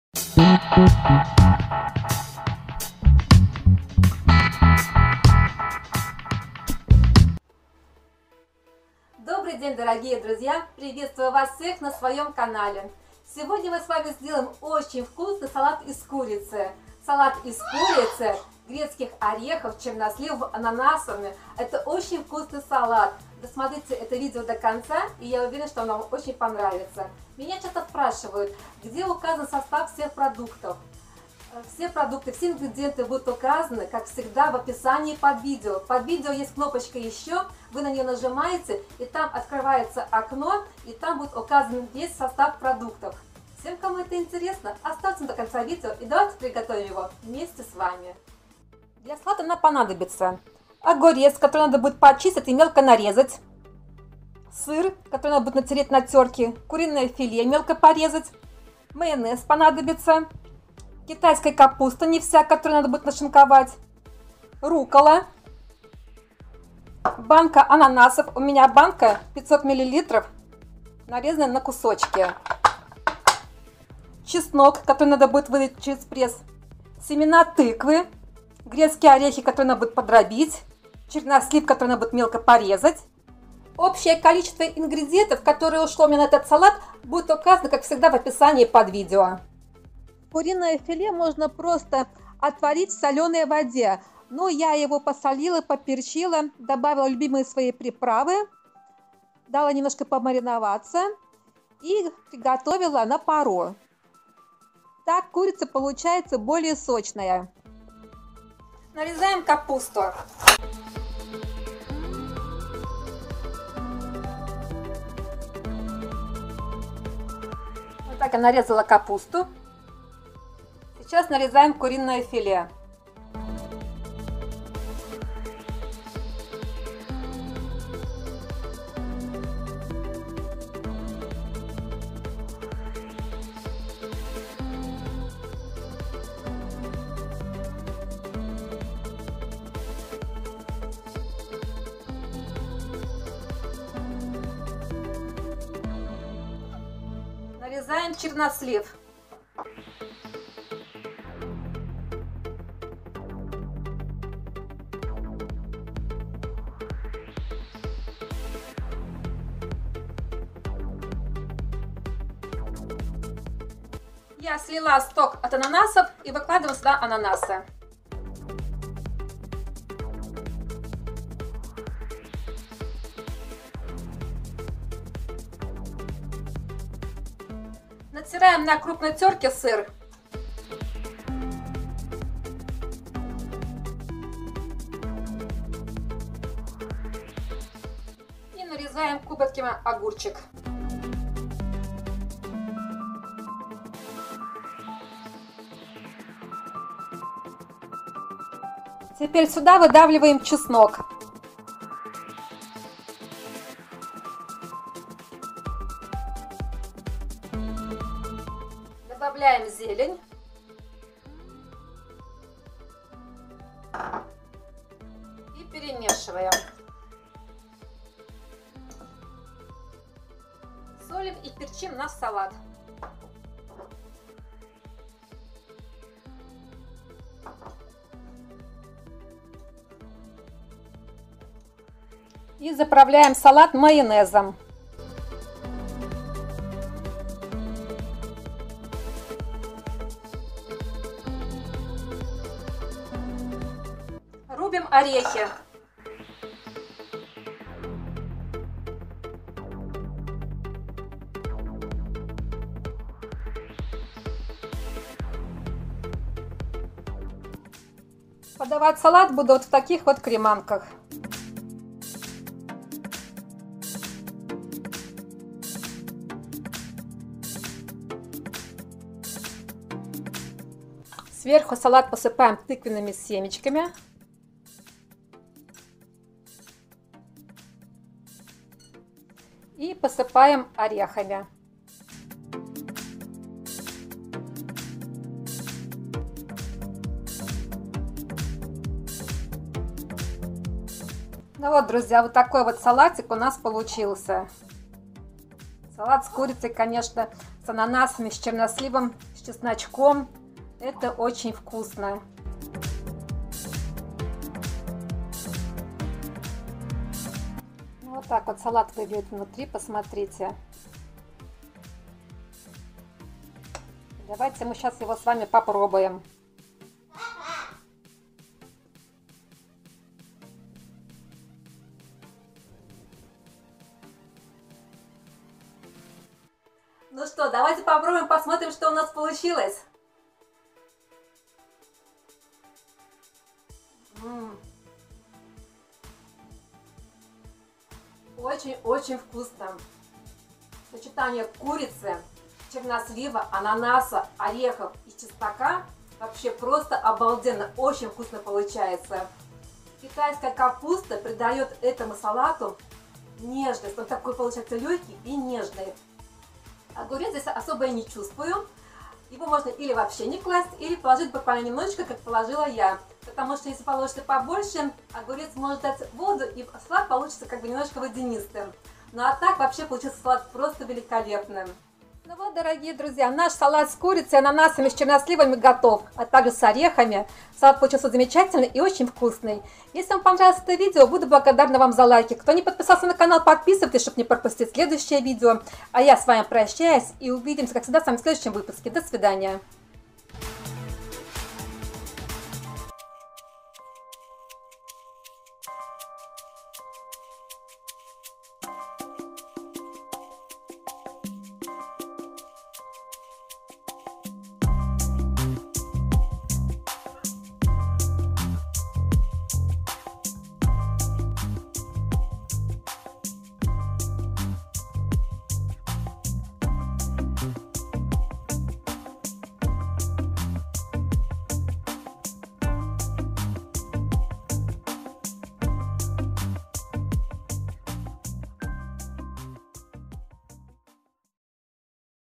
Салат с Курицей, Черносливом, Ананасом и т. д ./ Salad with Chicken, Prunes, Pineapple
Ингредиенты:

Куриная грудка - 300 гр.
Капуста китайская - 250 гр.
Свежий огурец - 150 гр.
Чернослив - 100 гр.
Грецкие орехи - 50 гр.
Сыр - 120 гр.
Чеснок - 3 дольки
Банка ананасов - 500 мл.
Майонез, зелень, соль, перец,
Тыквенные семечки - 10 гр.

Получается примерно 8 порций.

Ingredients:

Chicken Breast - 300 gr.
Chinese cabbage - 250 gr.
Fresh cucumber - 150 gr.
Prunes - 100 gr.
Walnuts - 50 gr.
Bank of pineapples - 500 ml.
Mayonnaise, herbs, salt, pepper,
Pumpkin seeds - 10 gr.

It turns out about 8 servings.

Мой хэштег в INSTAGRAM
Вы можете зарабатывать на YouTube на своих видео:
Подключайтесь к партнёрской программе :